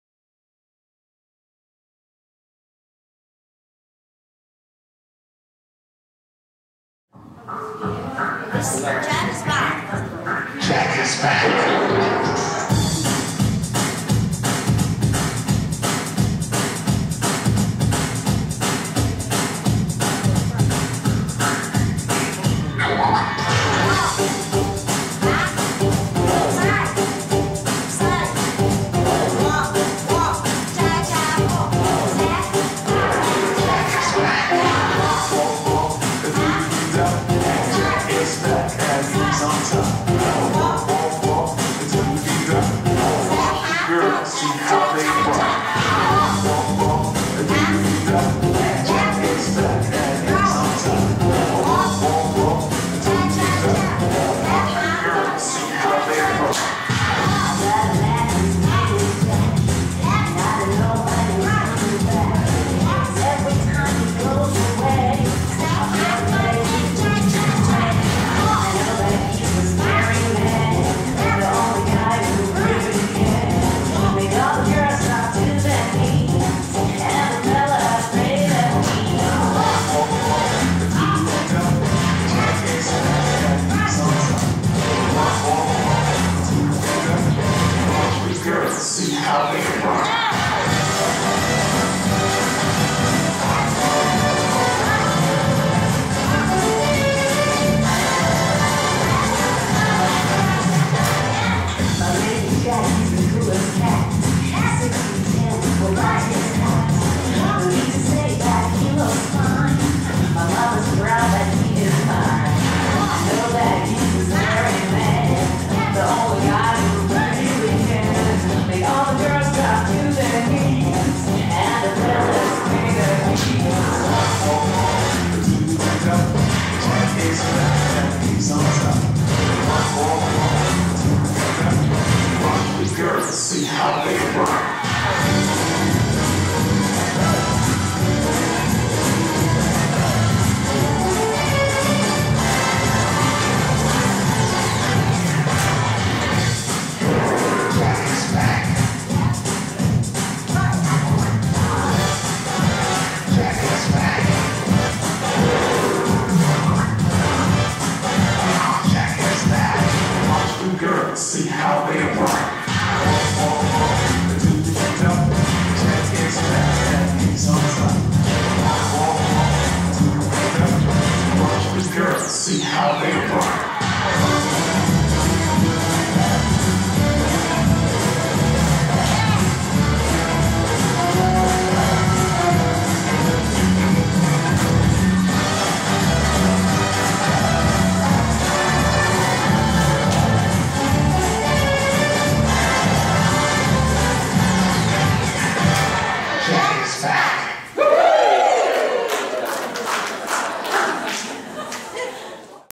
Jack Is Back (Senior Line Dance)
Julia Le Senior Line Dance Class, CPS Brampton ON.

Dance: Jack Is Back